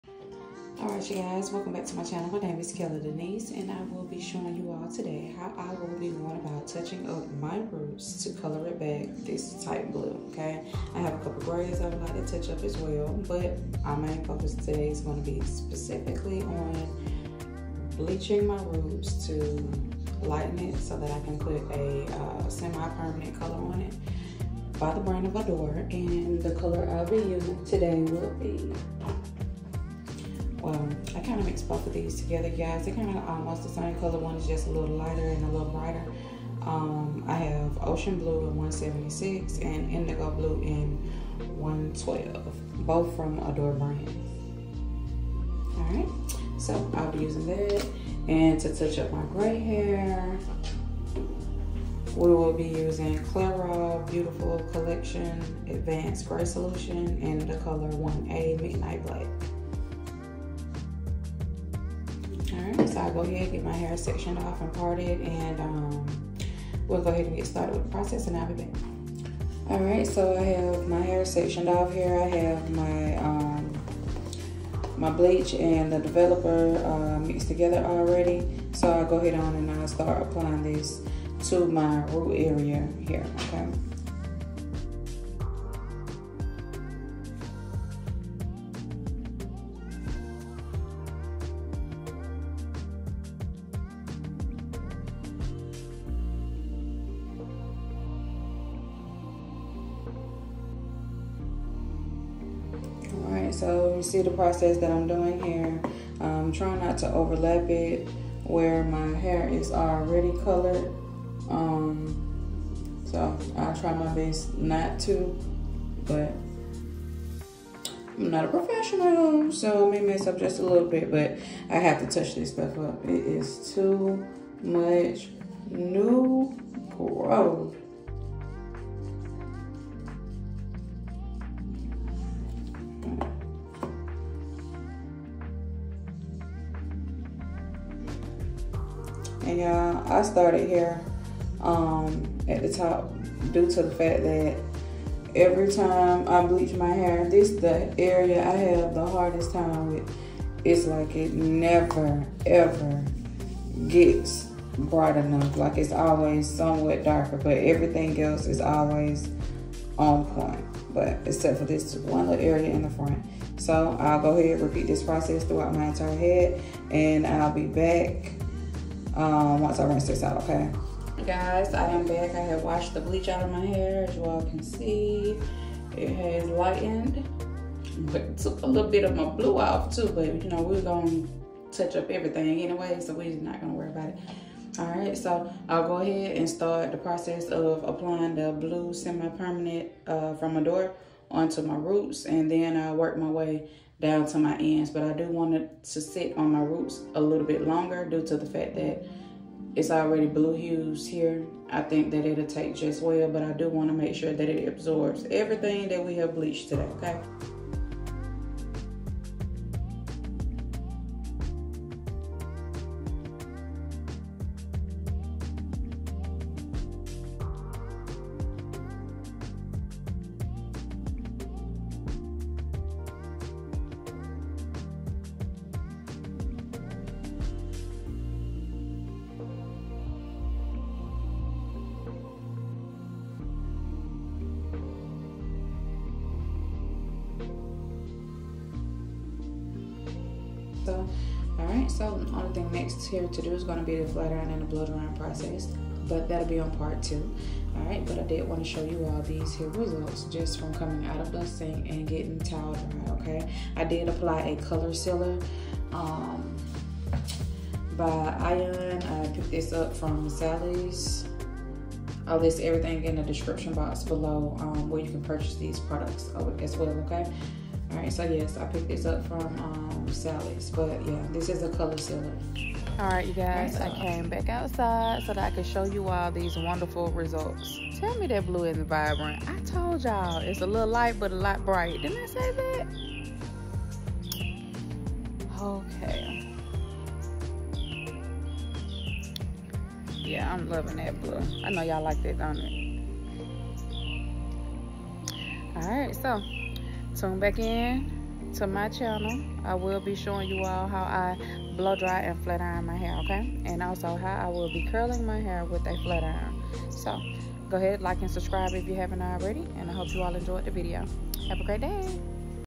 Dying my hair : Adore Indigo Blue & Ocean Blue FYP
Ready to transform your hair with bold, vibrant colors? 🌈 In this video, we're taking you through a stunning hair coloring journey using the Adore Semi-Permanent Hair Color! Known for its rich, long-lasting hues and gentle, nourishing formula, Adore is a favorite for anyone looking to add a pop of color to their hair without damage.

link to products:
Adore Indigo Blue
Adore Ocean Blue
Ion After Color Sealer